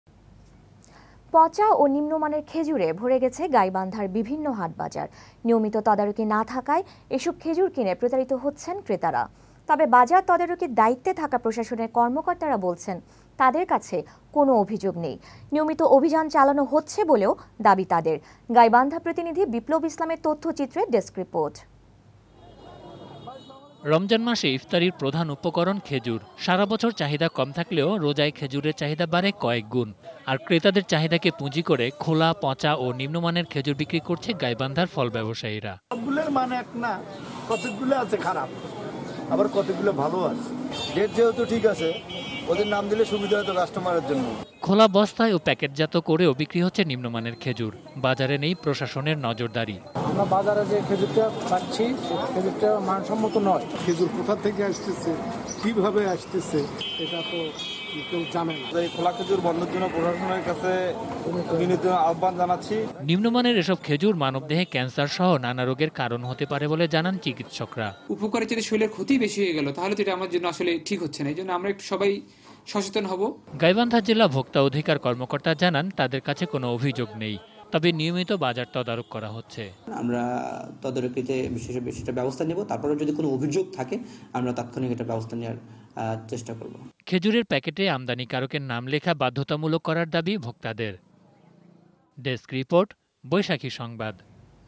পঁচা ও নিন্মমানের খেজুরে ভরে গেছে গাইবান্ধার বিভিন্ন হাট বাজার | Hat Bazar in Gaibandha | BD News
পঁচা ও নিন্মমানের খেজুরে ভরে গেছে গাইবান্ধার বিভিন্ন হাট বাজার | Hat Bazar in Gaibandha | BD News | Faruque | 10May19